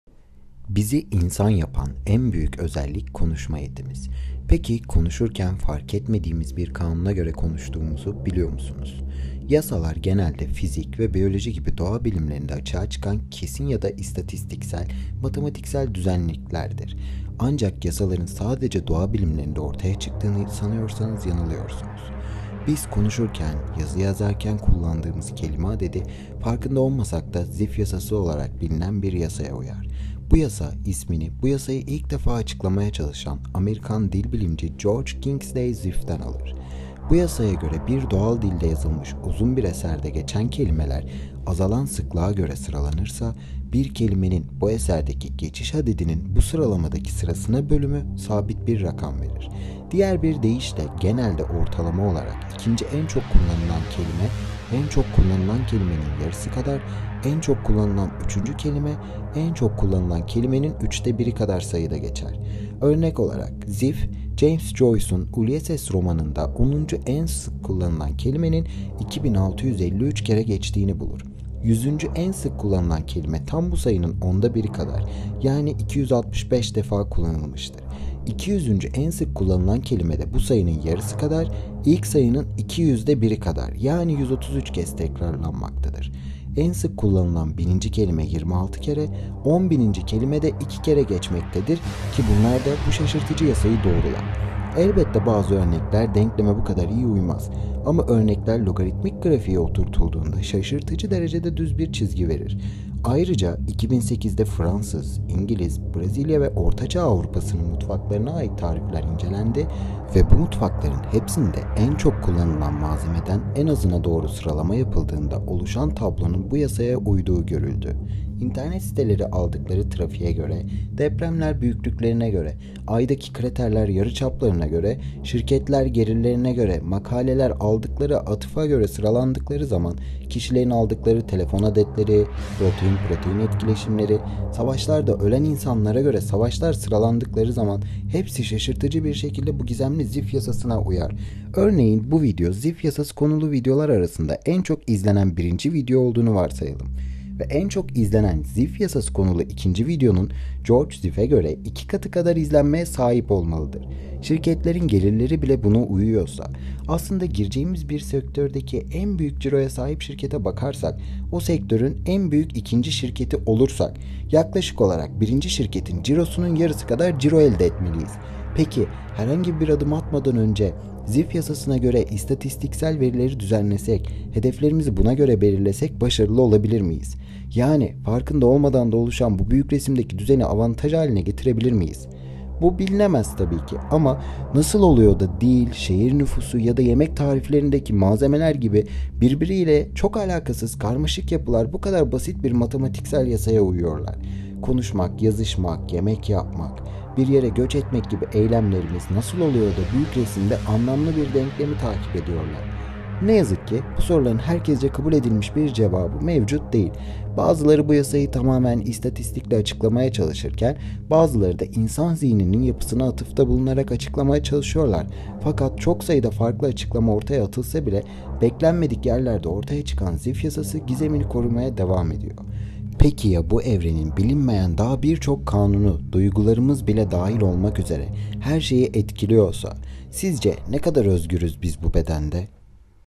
"GİZEMLİ ZİPF YASASI" SİZCE NE KADAR ÖZGÜRÜZ BİZ BU BEDENDE?
Bu gizemli yasaya göre, bir doğal dilde yazılmış uzun bir eserde geçen kelimeler, azalan sıklığa göre sıralanırsa, bir kelimenin bu eserdeki geçiş adedinin bu sıralamadaki sırasına bölümü sabit bir rakam verir. Diğer bir deyişle genelde ortalama olarak, ikinci en çok kullanılan kelime, en çok kullanılan kelimenin yarısı kadar, en çok kullanılan üçüncü kelime en çok kullanılan kelimenin üçte biri kadar  sayıda geçer. tüm diller de geçerli olmak üzere; depremler, nüfuslar,  şirket ciroları, yemek malzemleri vb. çoğu şey bu yasaya uymaktadır.